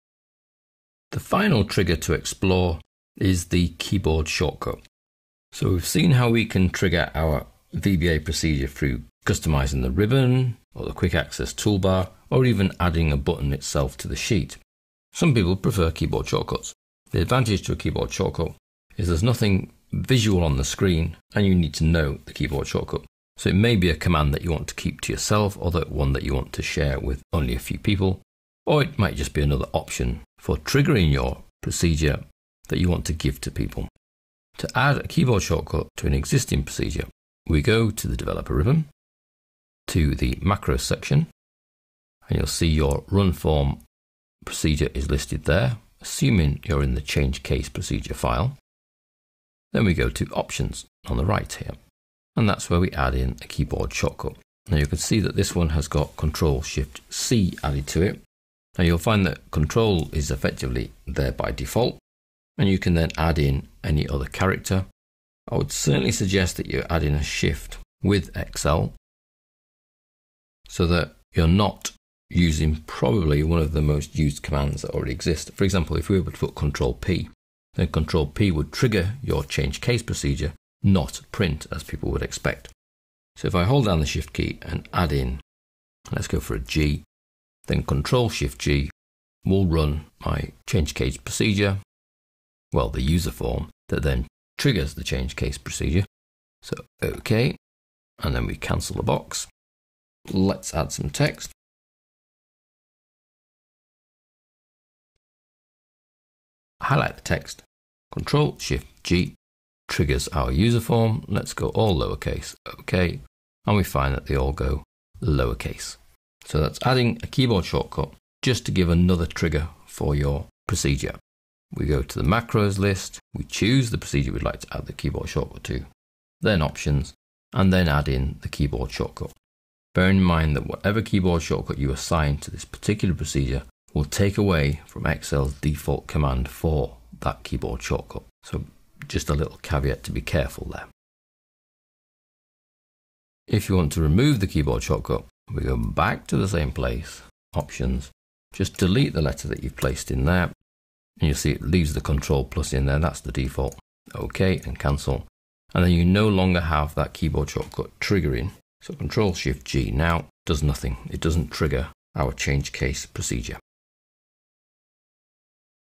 1104 Adding A Keyboard Shortcut Trigger
Visual Basic for Applications (VBA) in Excel 2010 to the Excel power user who is not yet a programmer. This article includes an overview of the VBA language, instructions on how to access VBA in Excel 2010, a detailed explanation of a solution to a real-world Excel VBA programming problem, and tips on programming and debugging